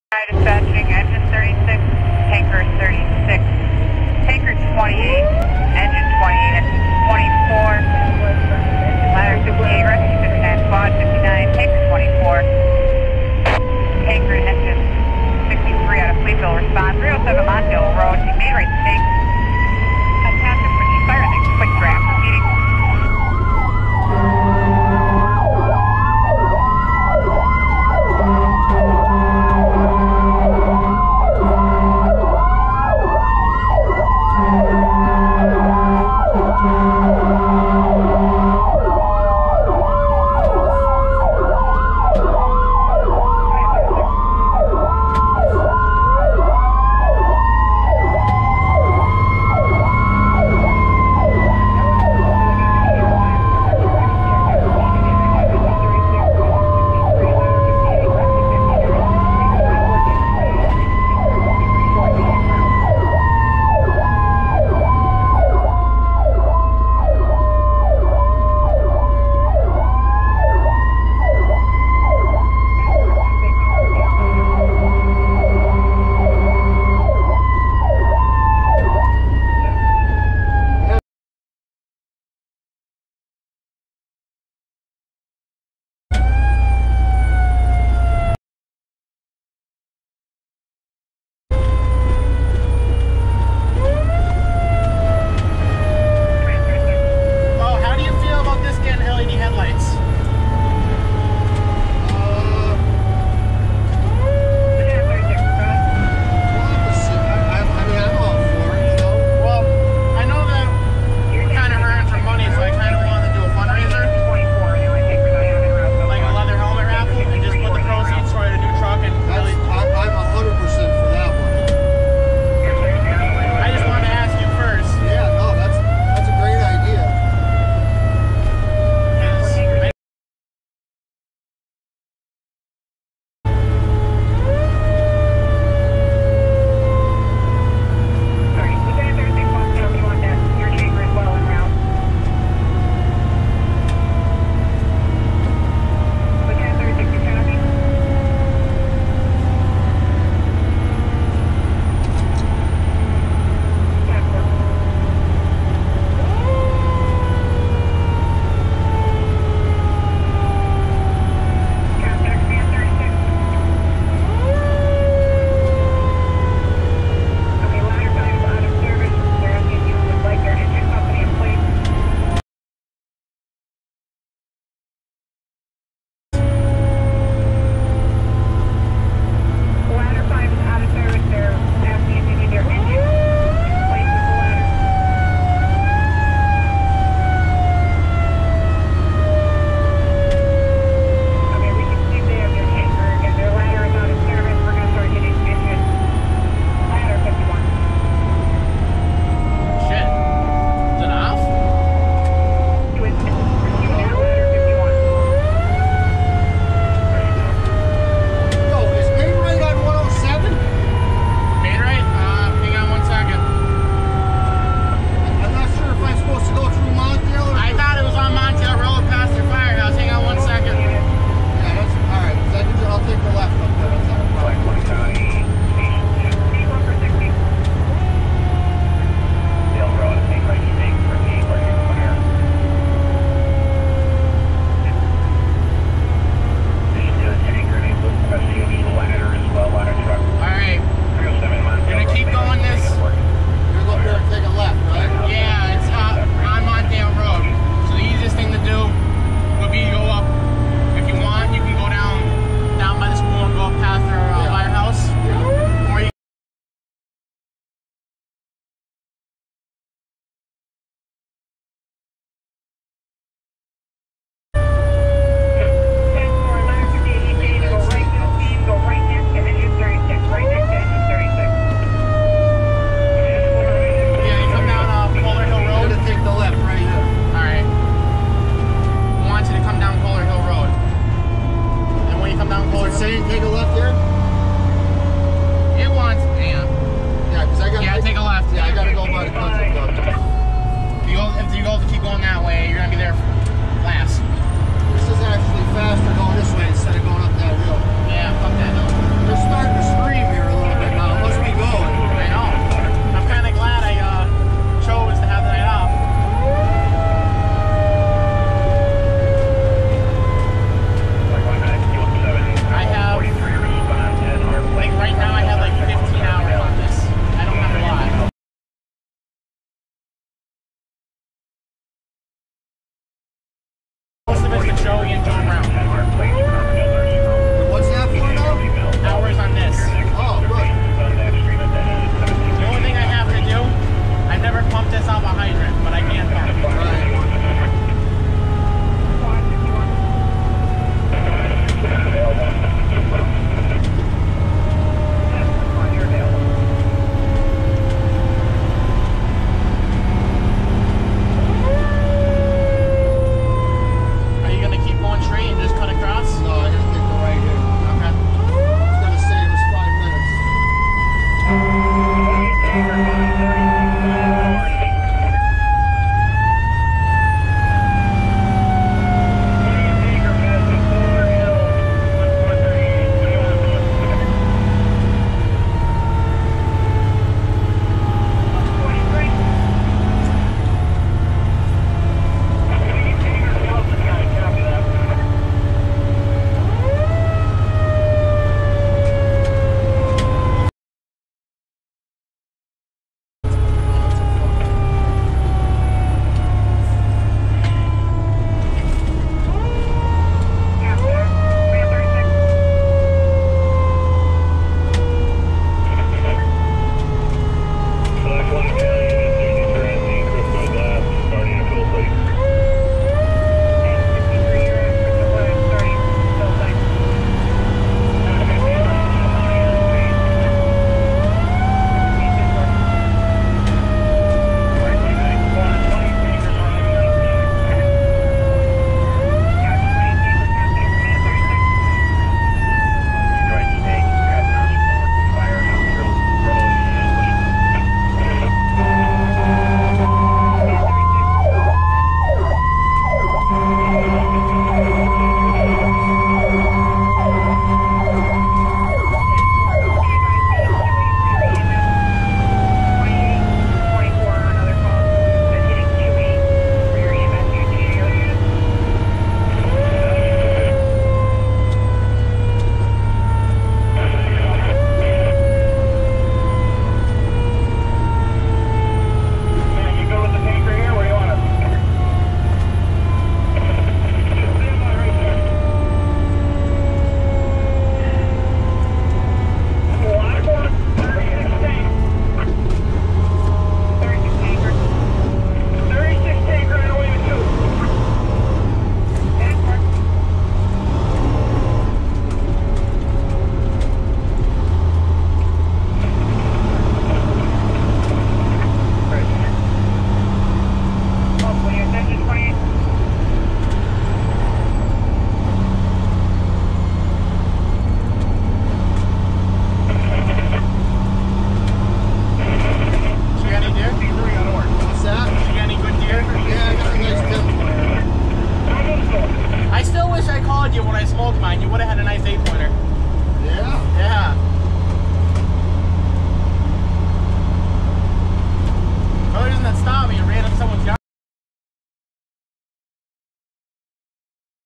*Ride Along* William Walker Hose Company Rescue 59 Responding.
Assigned Units: E36,TANKER36,L58,E28,TANKER28,E24,TANKER24,R59,SQ59,E63,TANKER63
Call Type: STRT
Radio Channel:
Address: 307 MONTDALE RD, 28
Muni: Scott Twp

Common Name: MAID RITE STEAK SCOTT TWP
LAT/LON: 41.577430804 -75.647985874
Closest Intersection: JOE LICK RD, BUSHKO'S HILL RD / HOPFER RD
Call Time: 11/29/2021 17:19:20
Nature of Call: fire in the quick draft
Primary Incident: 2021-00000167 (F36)